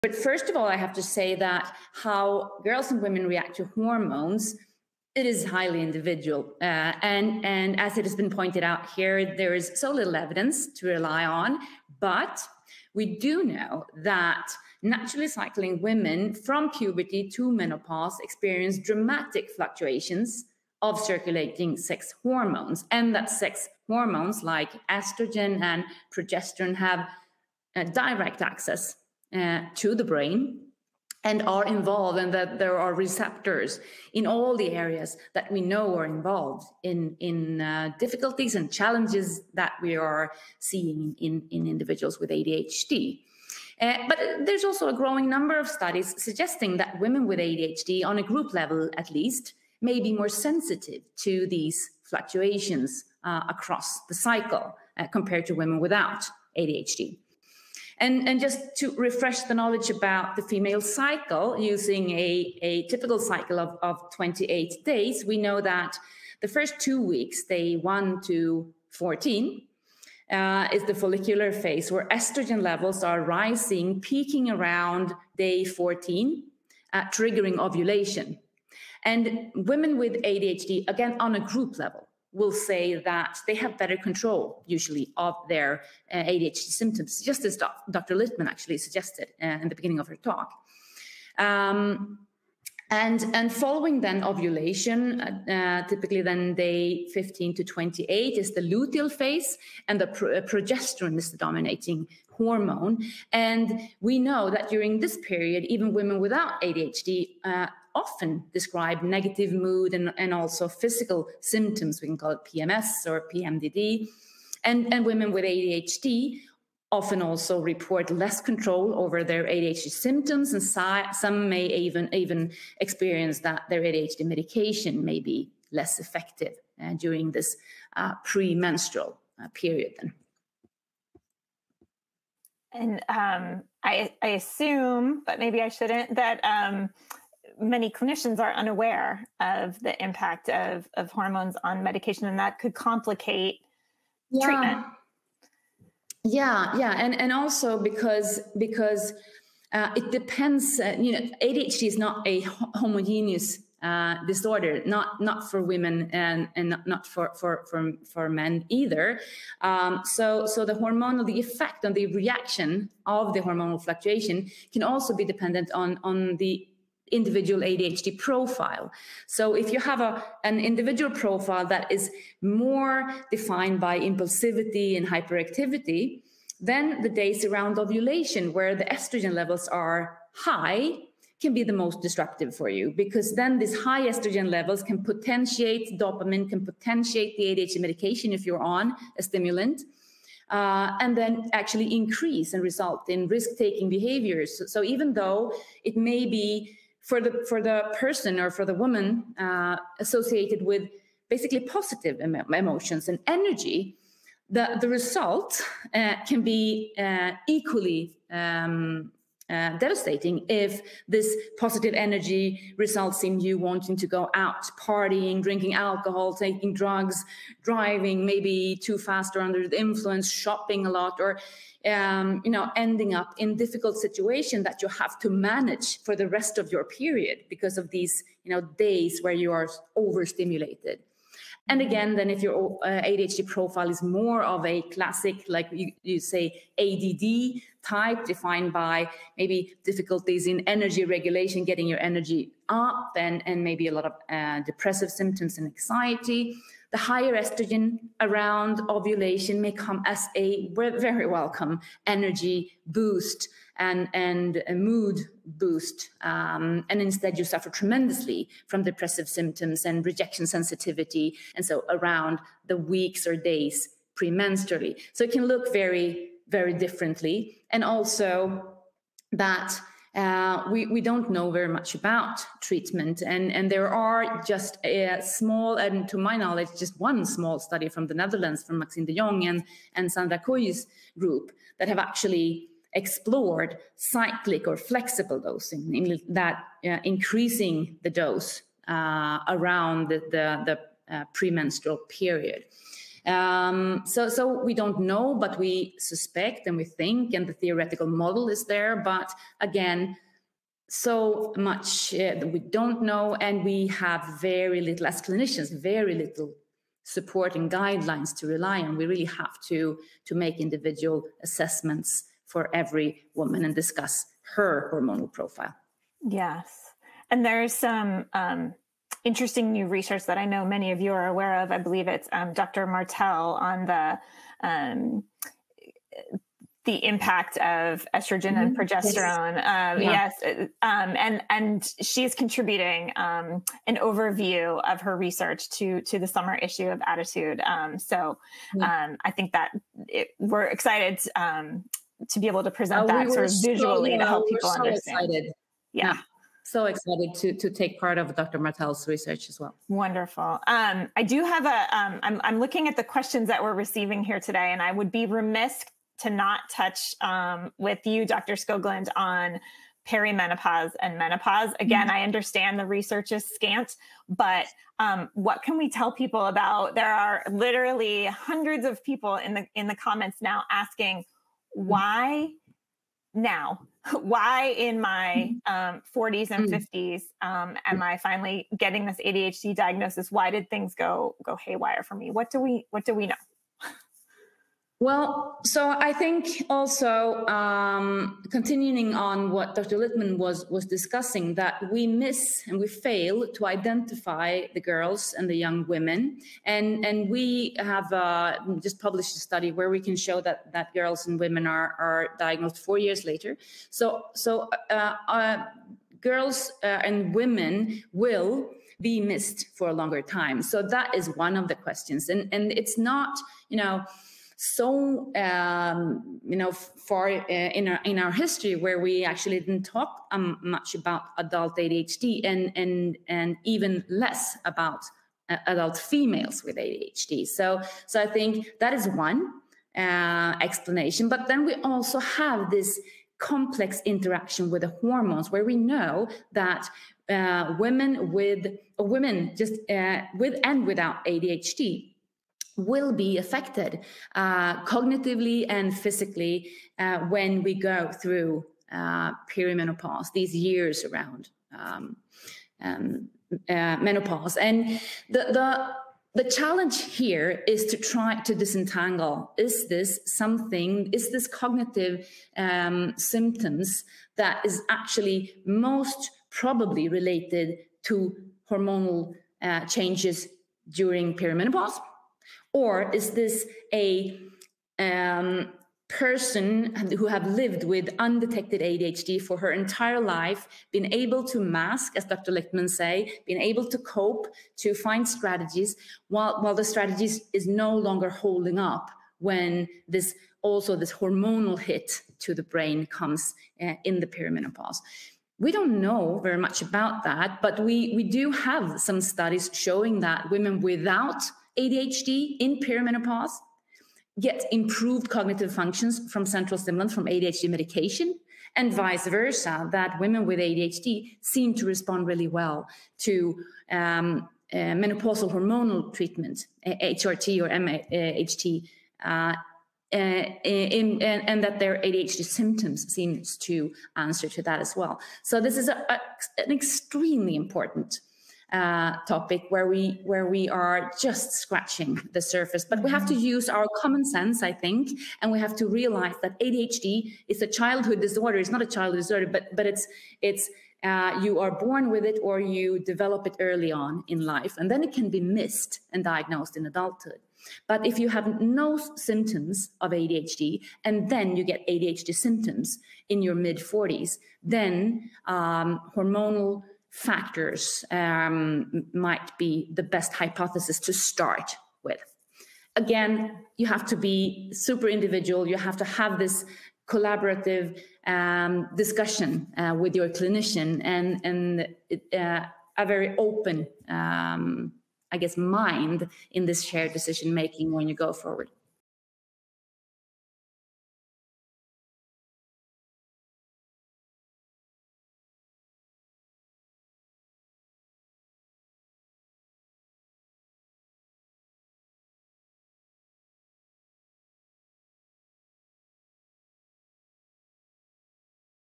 The Menstrual Cycle and ADHD (with Lotta Skoglund)
"Women with ADHD may be more sensitive to fluctuation across the cycle compared to women without." - Skoglund

Women with ADHD experience significant hormonal fluctuations from puberty through menopause, as sex hormones like estrogen and progesterone directly affect brain regions linked to ADHD’s challenges. Dr. Skoglund has been studying the effects of hormonal fluctuations on women with ADHD across the reproductive cycle. This is an area of research that has long been neglected. Dr. Skogland explains how do ADHD symptoms change during the menstrual cycle and how that that may impact a girls' chance of diagnosis.

In this ADDitude roundtable, leading experts discuss the lived experiences of women with ADHD, the need for more research, and the importance of an open exchange of ideas to increase understanding and support. This roundtable was originally broadcast on March 4, 2025.